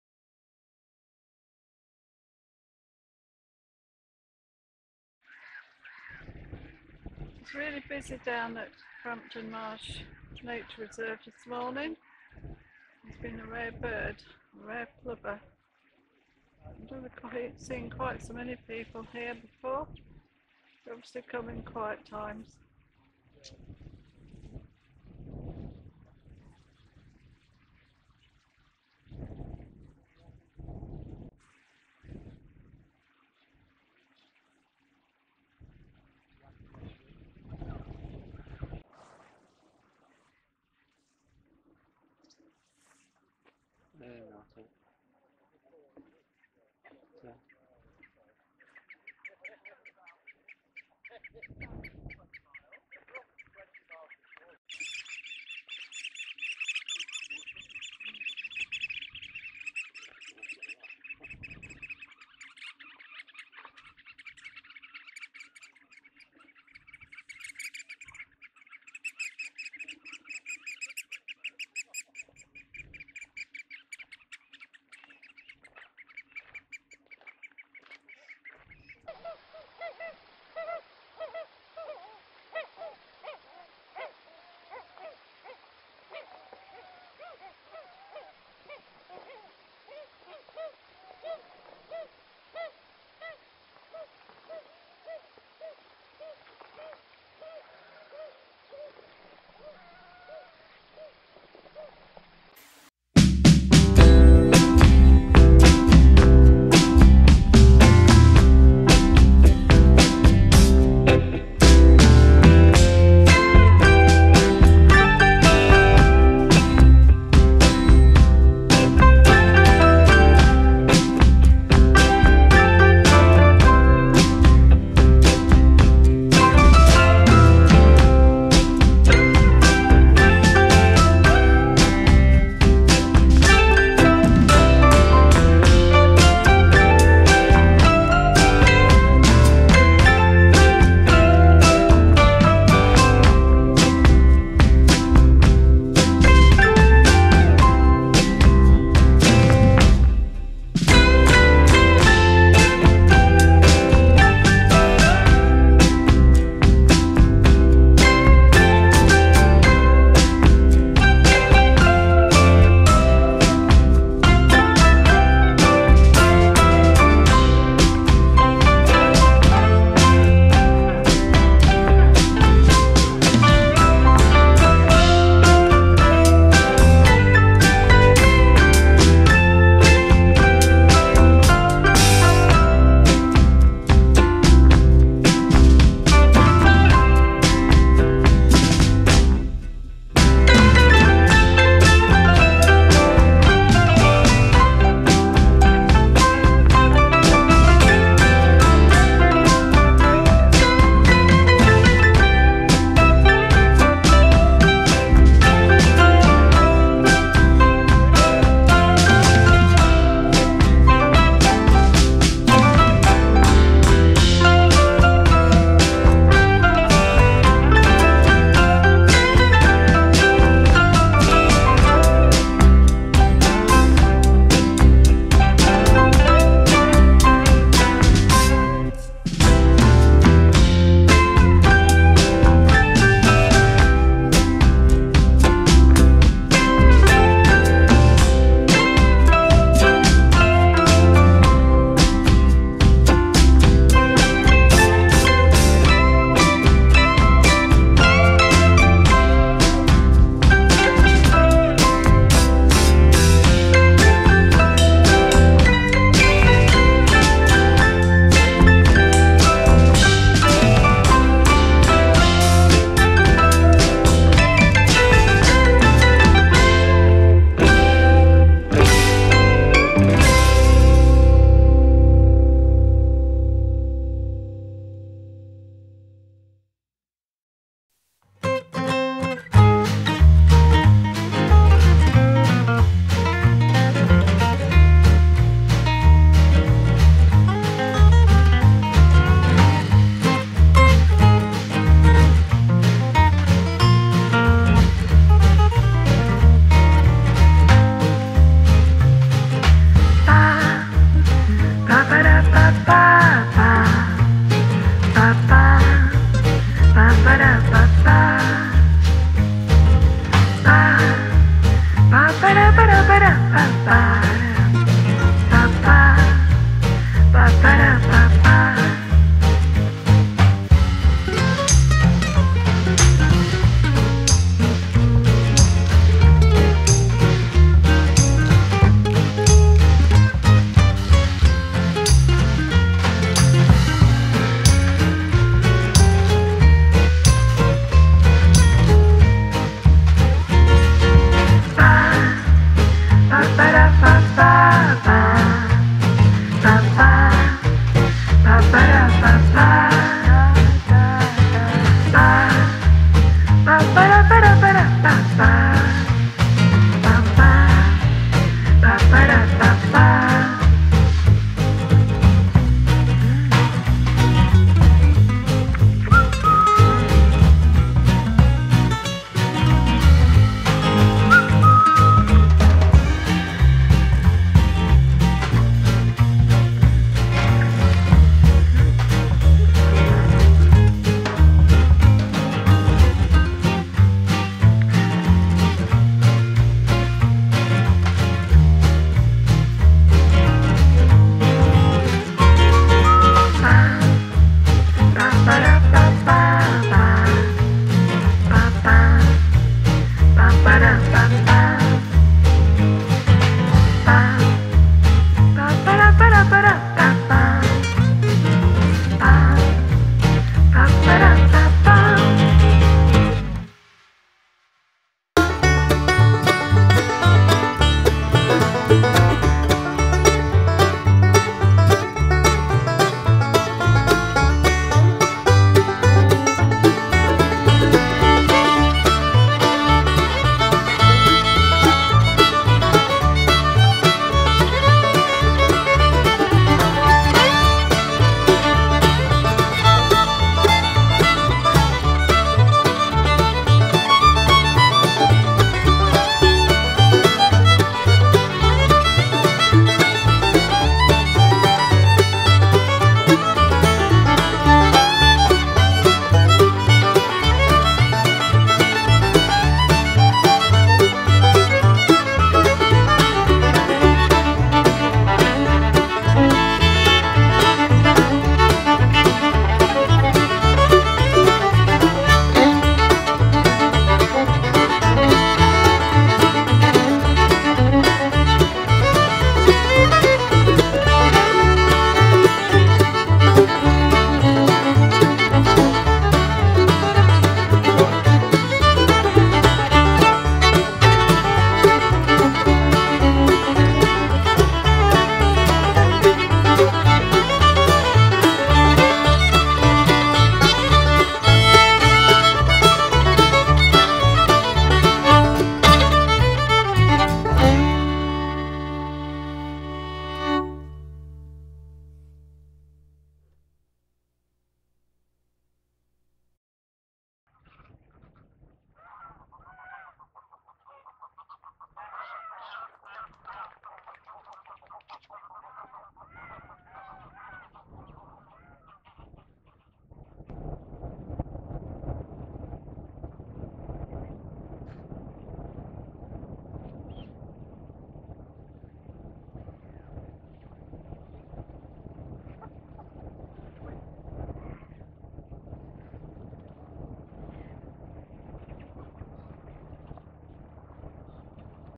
RSPB, A Lovely Walk at Frampton Marsh Nature Reserve, Oyster Catchers, Dunlin`s, Spoonbills chasing.
RSPB - A Lovely Walk at Frampton Marsh Nature Reserve, Oyster Catchers, Dunlin`s, the Spoonbills were chasing each other with sticks, geese, ducks.
Different birds come and go with the seasons, which makes our walks interesting.